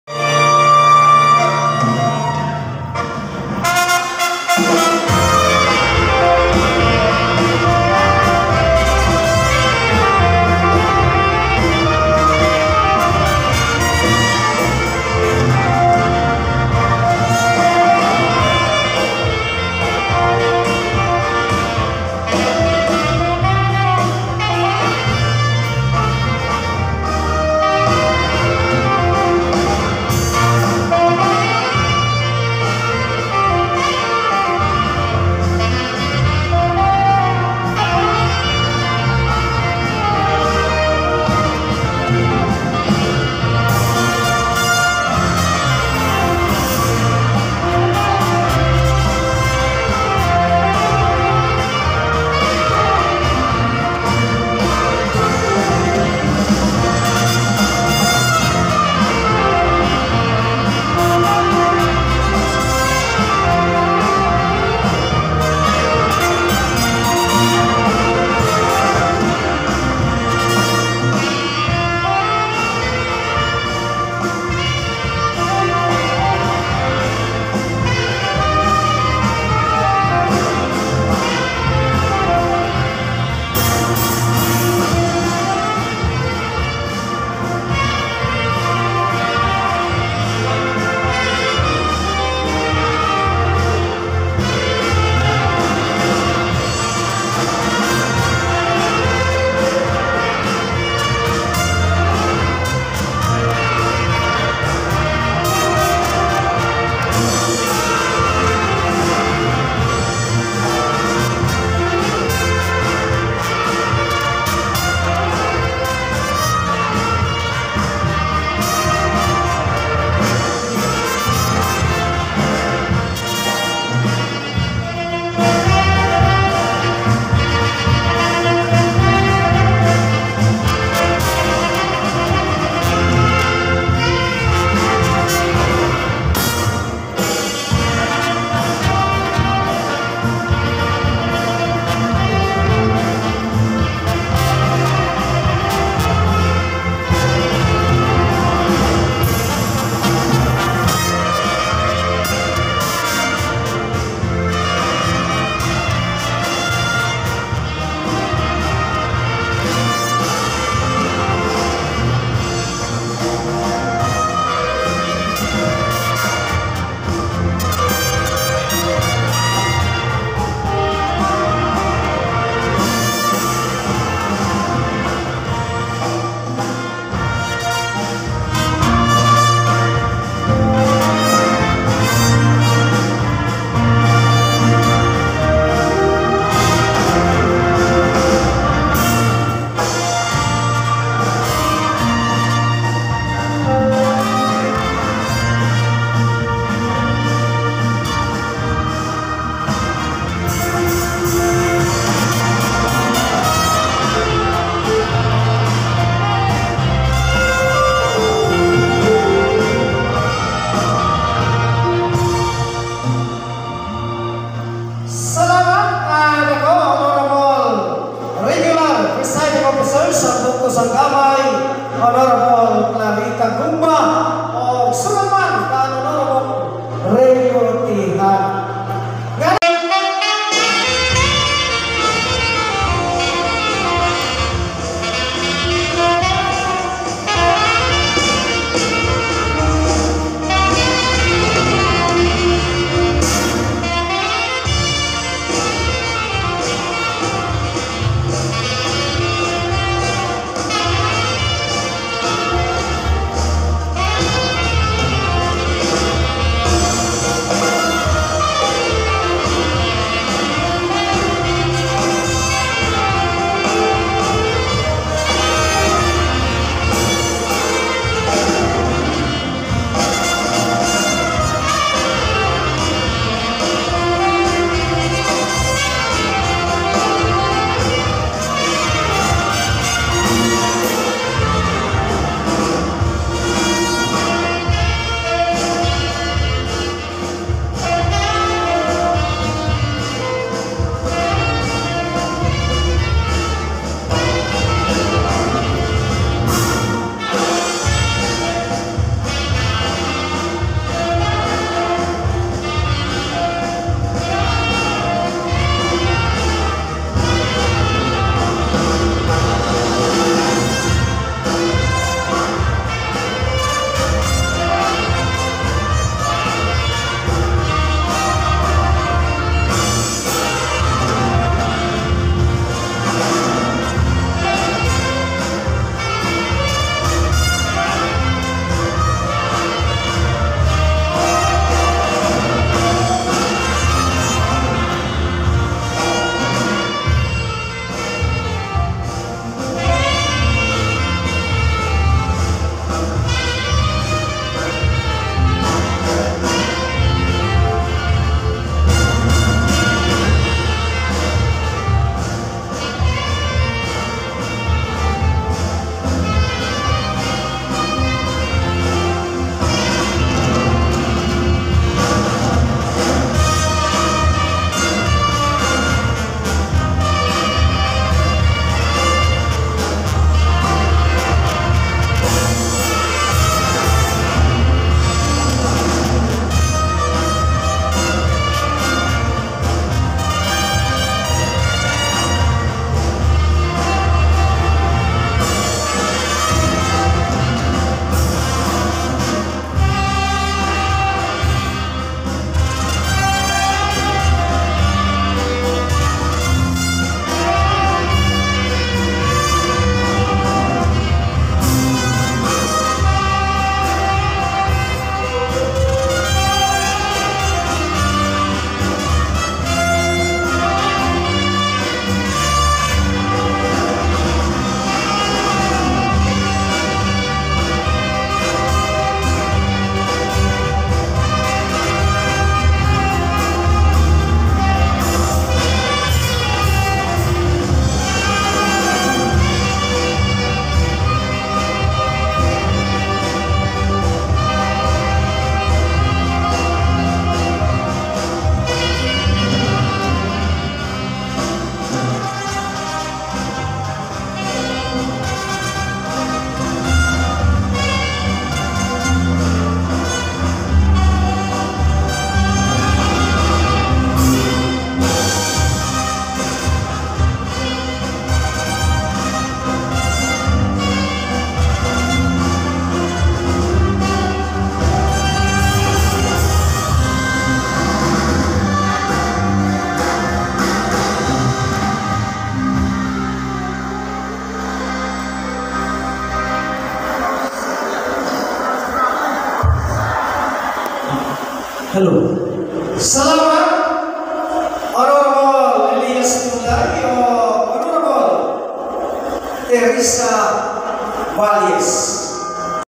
KURATSA VICE MAYOR OF GAMAY VS SB MEMBER 🎉 MAPANAS TOWN FIESTA
Mapanas Northern Samar town fiesta vesper celebrations 2024.